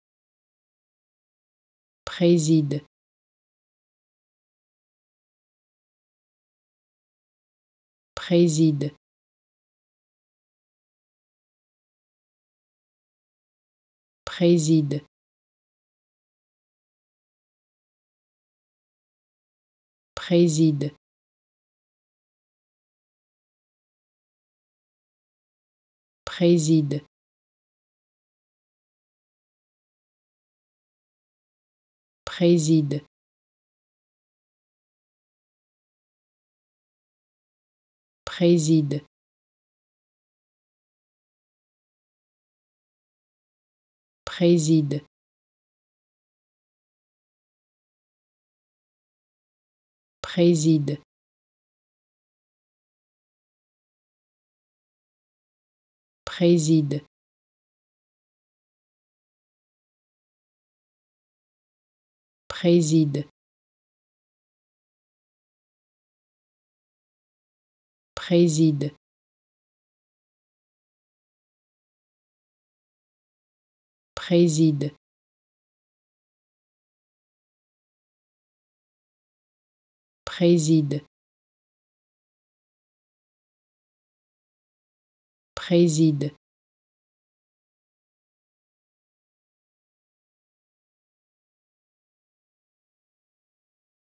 How to Pronounce ''Préside'' (President) Correctly in French
Learn how to say and properly pronounce ''Préside'' in French with this free pronunciation tutorial.